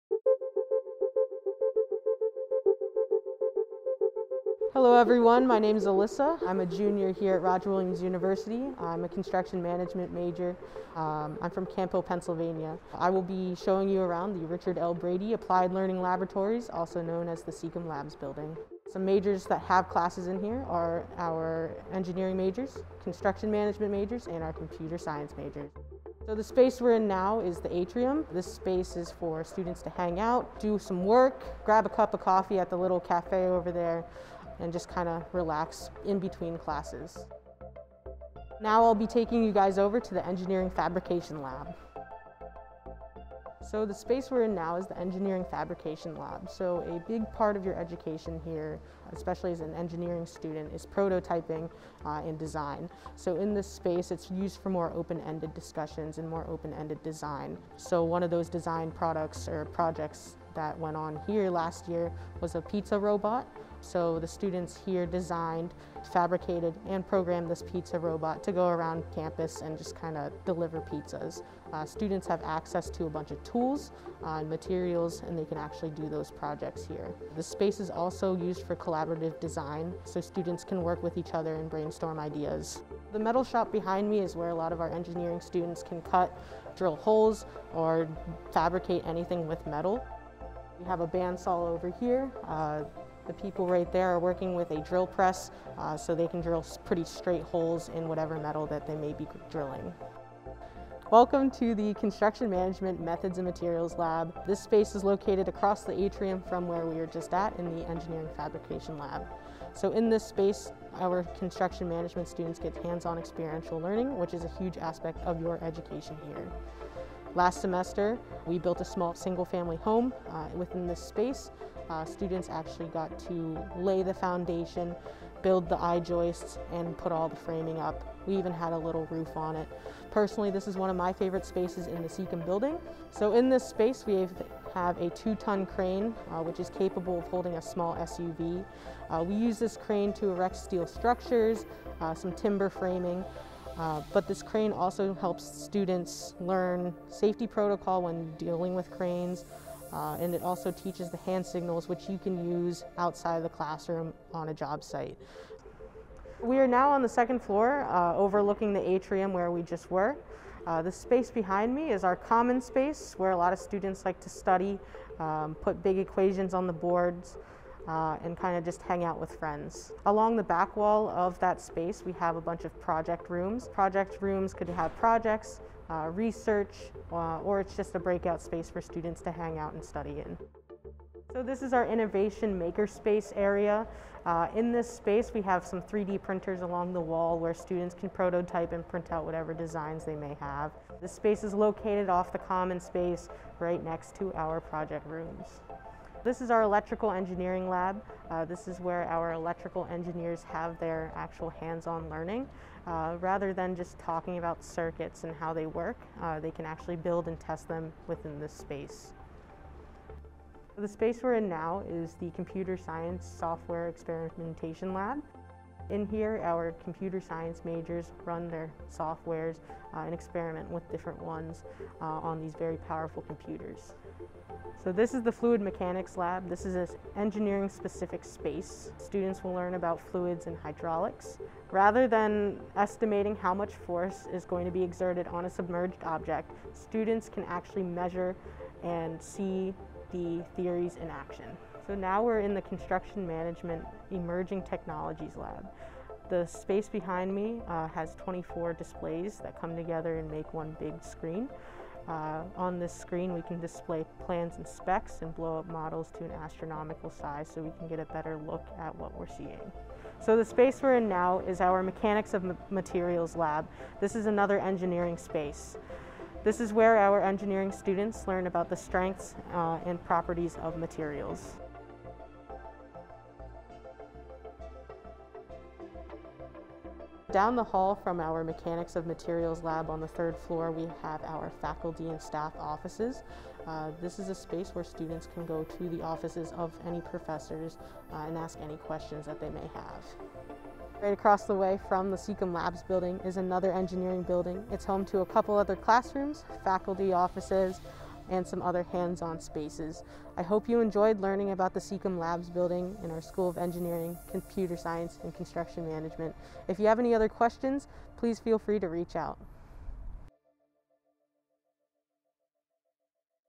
Take a Look Inside the School of Engineering, Computing, and Construction Management
Join student tour guide, Alyssa, on an overview of what Roger Williams University offers through our School of Engineering, Computing, and Construction Management. Hear about the different majors, technology, and opportunities available to all undergraduate students.